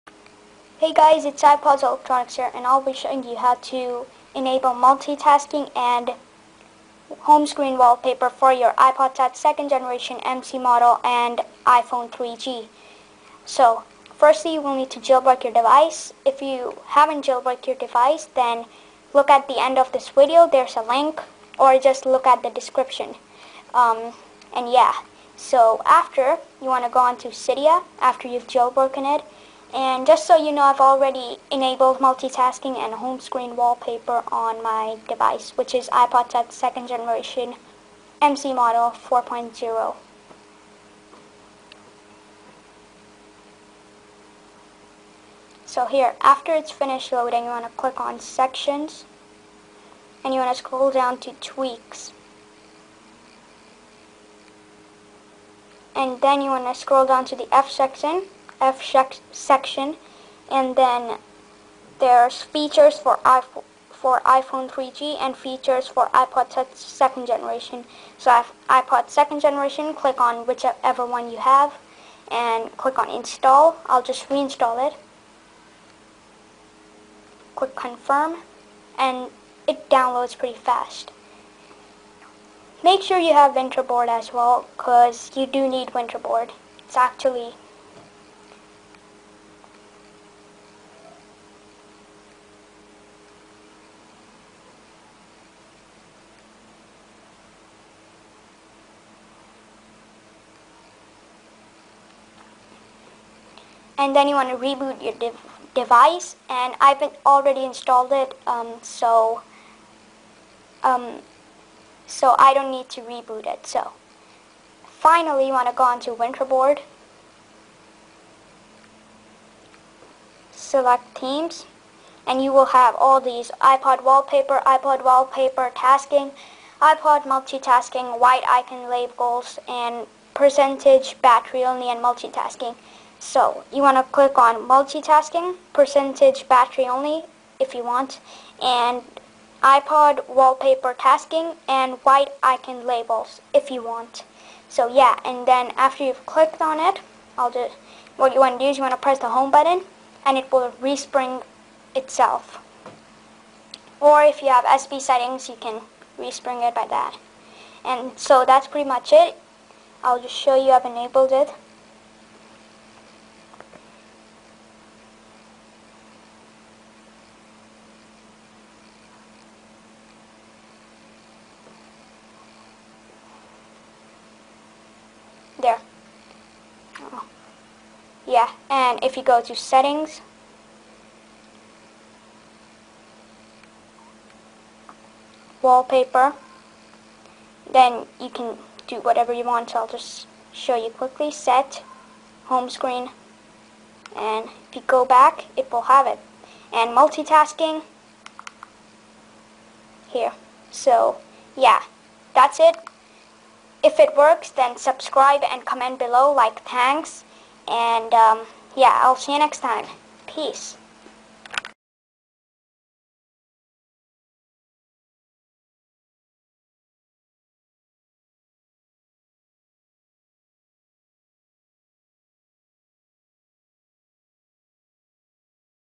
Enable Multitasking & HomeScreen Wallpaper on iPod Touch 2G (MC) & iPhone 3G - iOS 4
No source required.

THIS IS NOT A COPYRIGHT VIDEO

Thanks to:
IpodAppsWorld